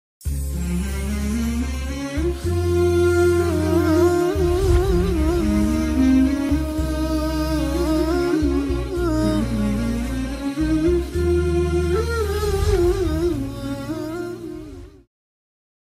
Tri çelësat
Duaja është çelësi i rrahmetit.
Abdesi është çelësi i namazit.
Namazi është çelësi  i xhennetit